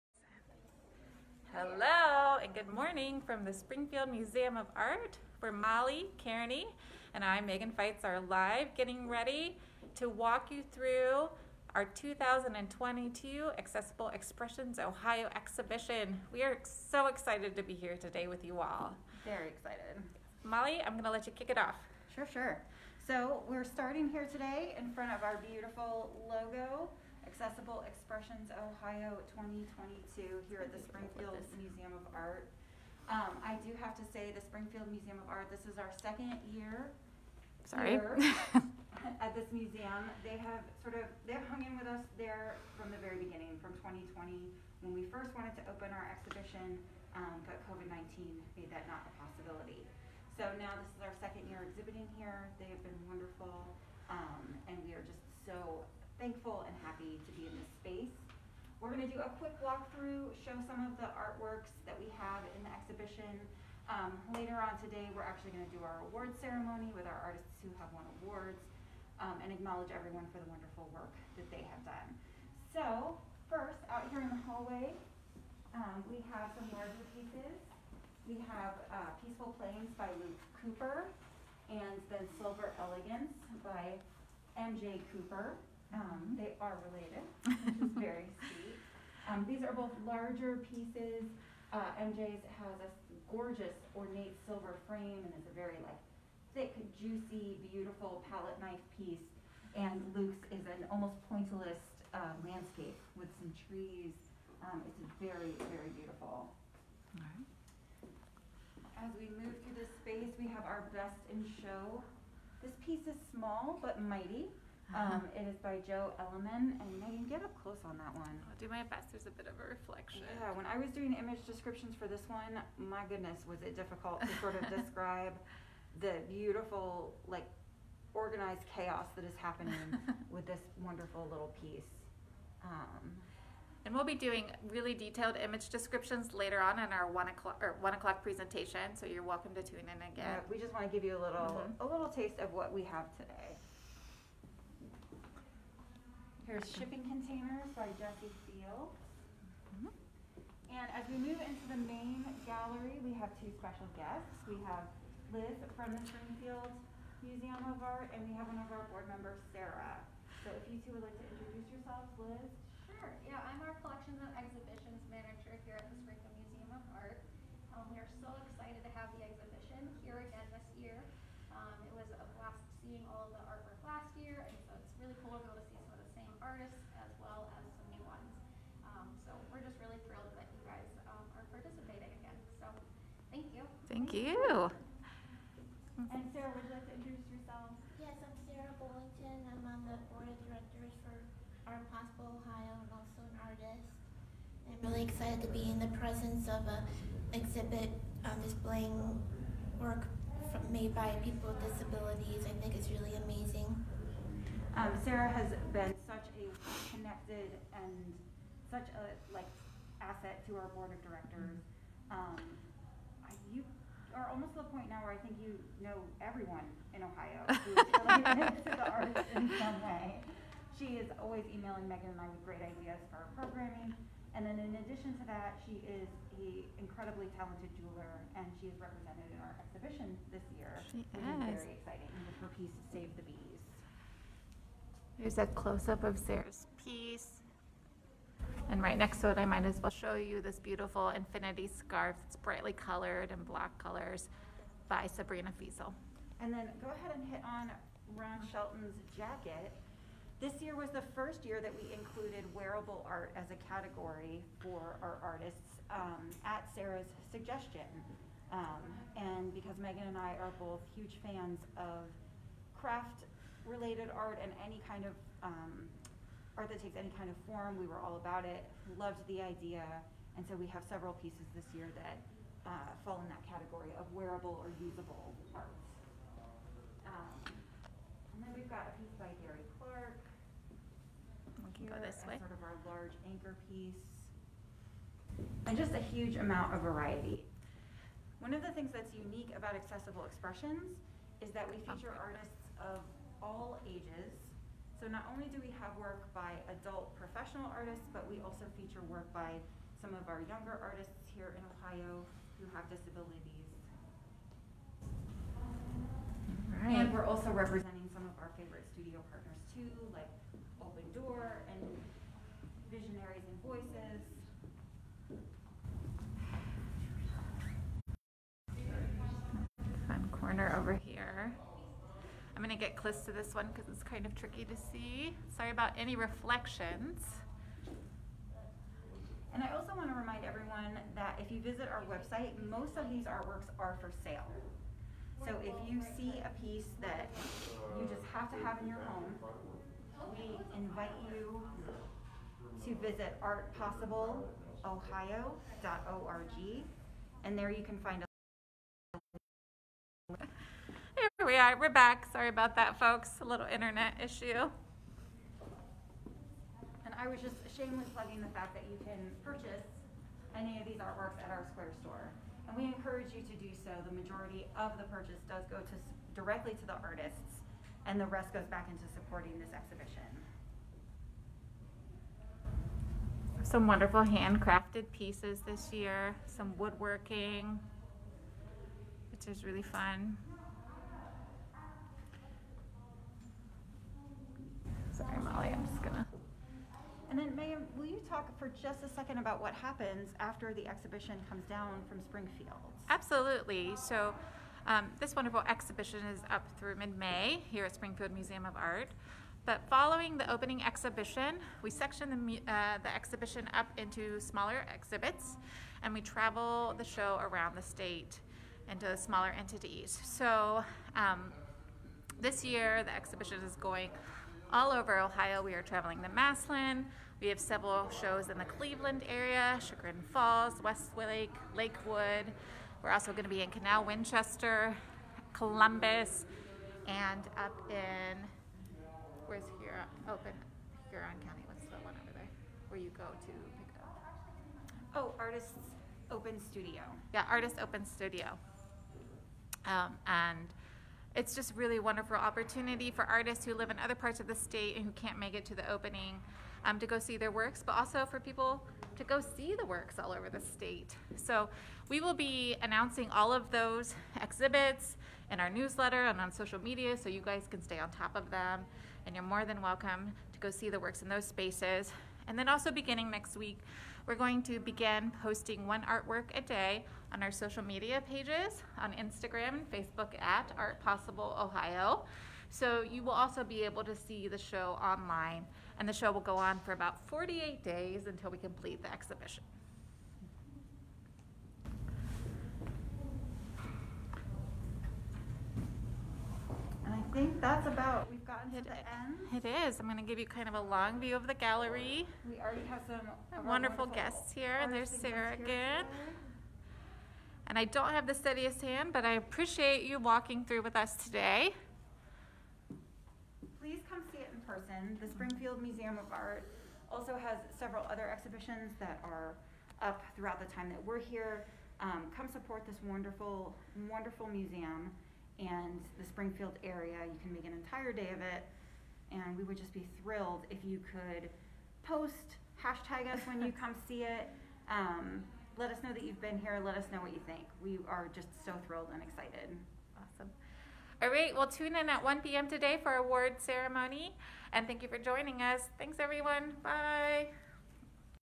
Accessible Expressions Ohio 2022 Virtual Walkthrough at the Springfield Museum of Art
Join Executive Director, Molly Cairney, and Director of Programs, Megan Fitze, as they walk you through the Accessible Expressions 2022 Exhibition held at the Springfield Museum of Art!

Apologies some sideway images at the beginning, we eventually worked out the best way to hold the camera!